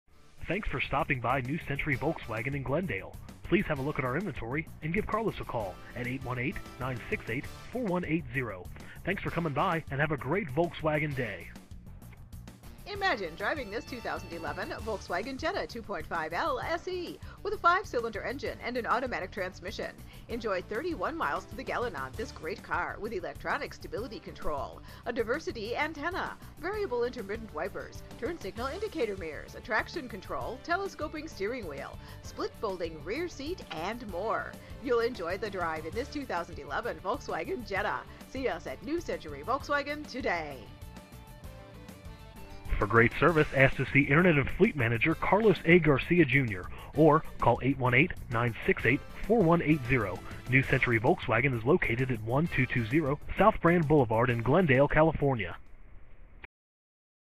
2011 Volkswagen Jetta New Century Volkswagen Glendale, CA 91204
Volkswagen Jetta Check out this Silver Volkswagen Jetta Sedan, equipped with a 5 Cylinder engine and an automatic transmission. Enjoy an exceptional 31 miles to the gallon on this great car with features like CD Player, Security System, Front Bucket Seats, Tilt Steering Wheel, Air Conditioning, Tachometer, Power Windows, Power Steering, and much more. Have peace of mind in this Volkswagen Jetta. See us at New Century Volkswagen today!
MILES: 39,210
VIN:3VWDZ7AJ2BM343672

1220 South Brand Boulevard
Glendale, California 91204